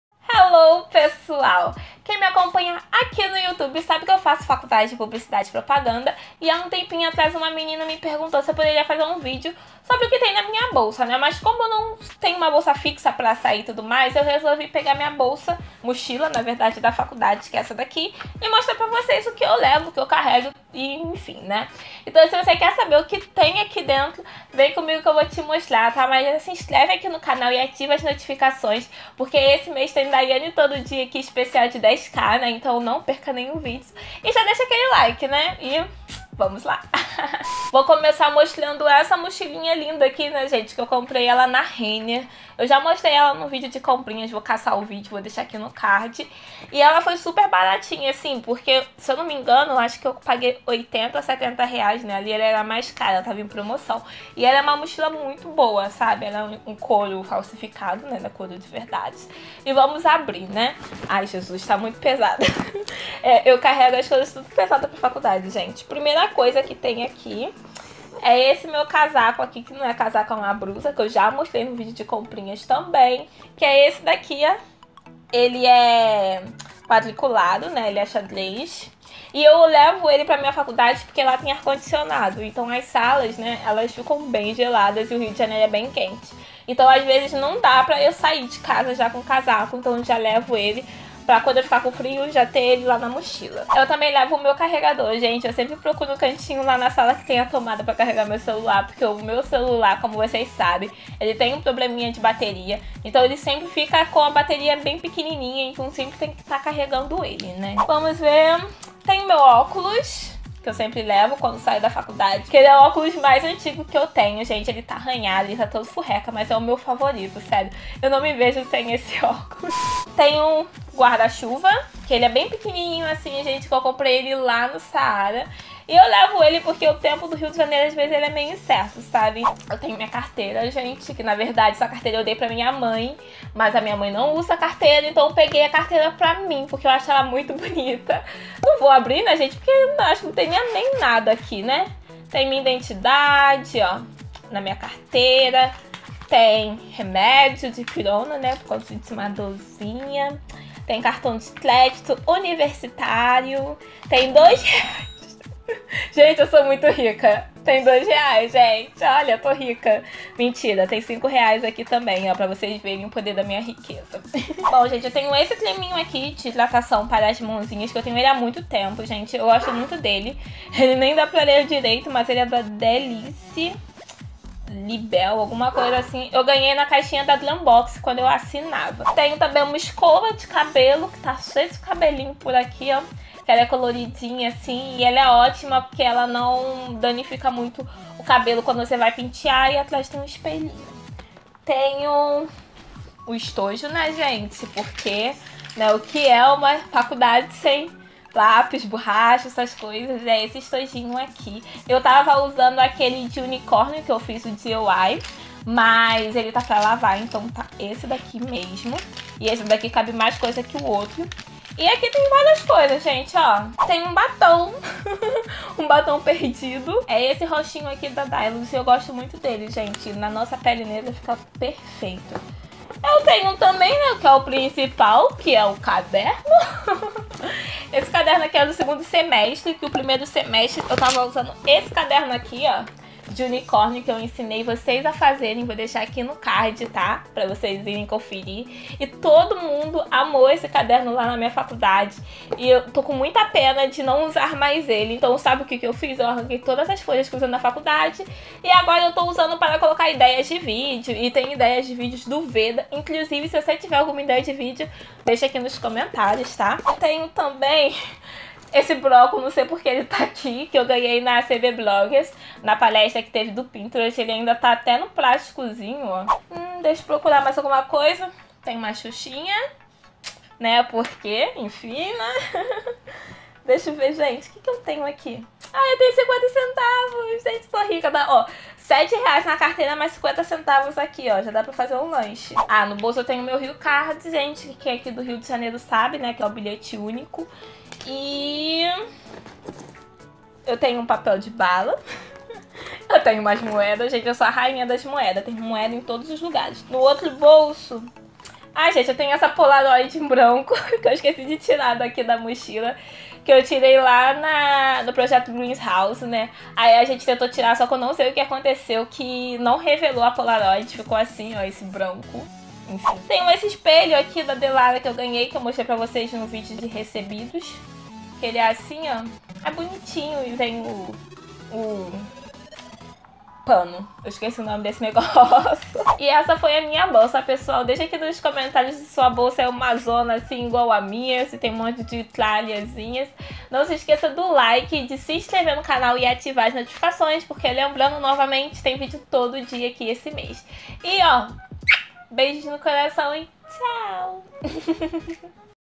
O QUE TEM NA MINHA BOLSA DA FACULDADE? | DAYANE NASCIMENTO
Nesse vídeo estou mostrando o que tem na minha bolsa da faculdade e estou mostrando tudo viu! Desde o caderno, até as moedinhas perdidas! hahah Vem conferir ♥

Edição: Sony Vegas

Quer saber mais sobre meu cabelo, se é aplique ou não? Confere o vídeo que eu fiz que contei tudo!

Beijinhos ♥